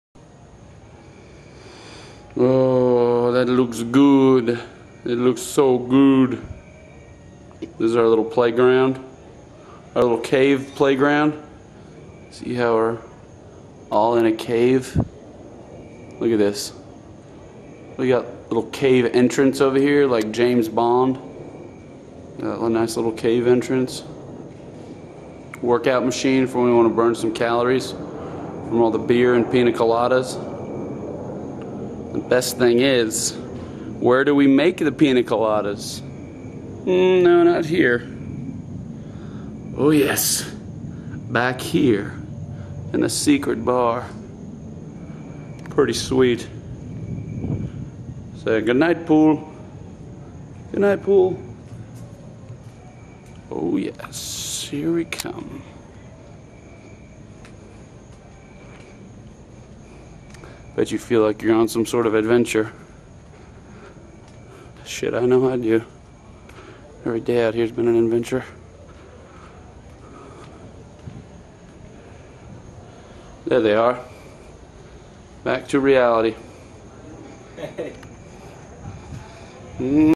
Acapulco Spring Break 2010 - playground
Living the good life in Acapulco, Mexico during Spring Break 2010